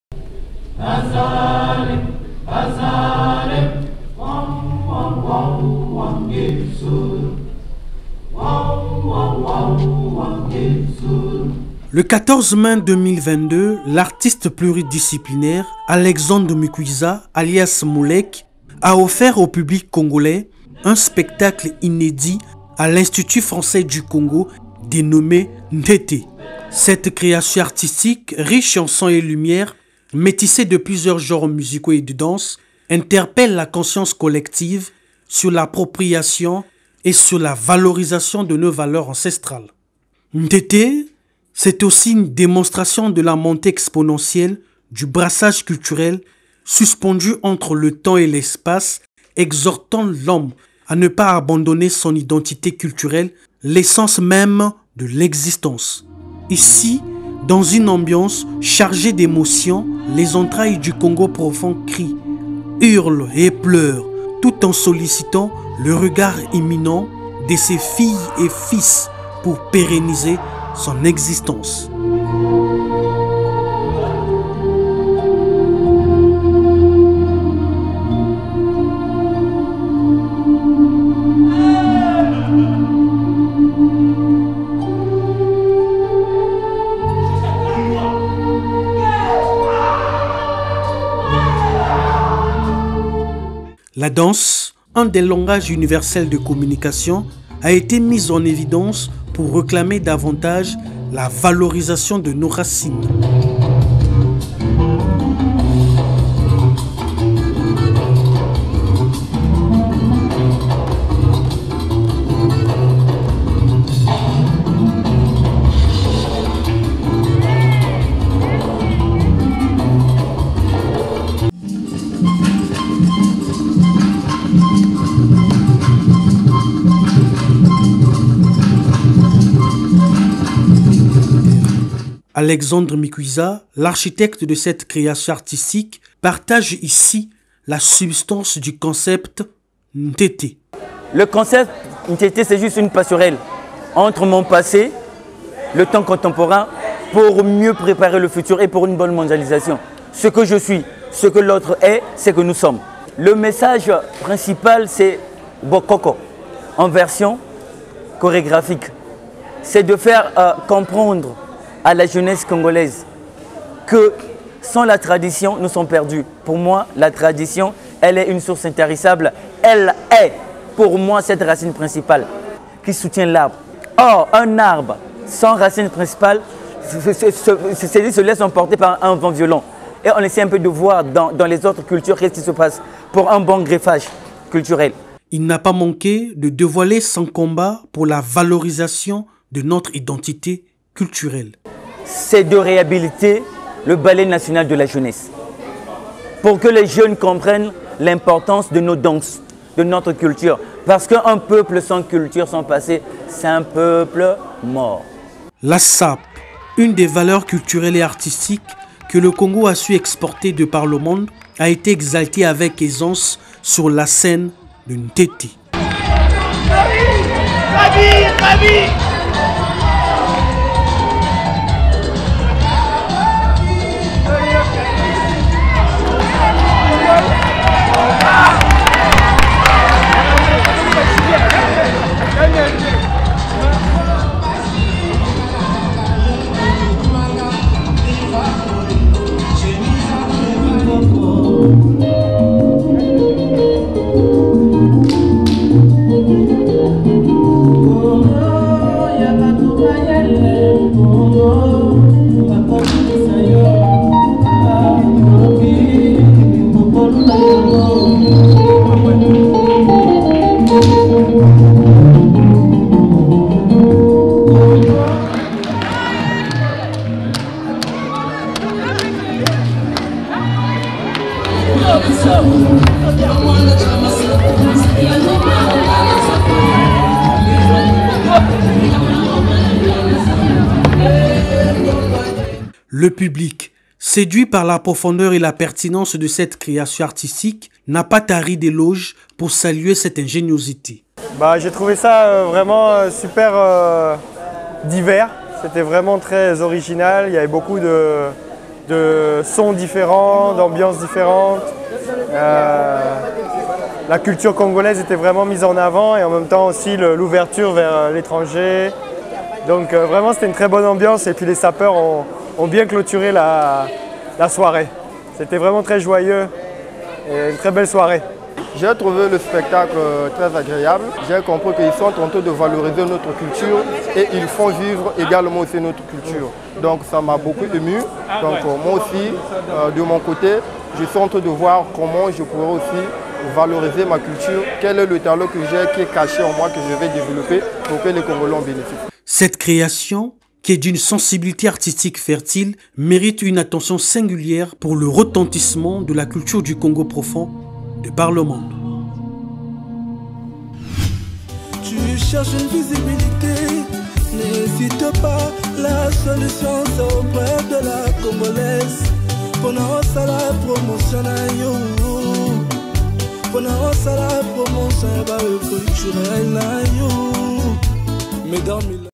NTETE POUR LA VAORLISATION DE LA CUTURE DU CONGO PROFOND
Ce spectacle qui interpelle la conscience collective sur l’appropriation et sur la valorisation  de nos valeurs ancestrales est d’une sensibilité artistique fertile.
 Alexandre Mikouiza , l'architecte de cette création artistique  a fait des révélations inédites sur son combat.
A découvrir dans ce reportage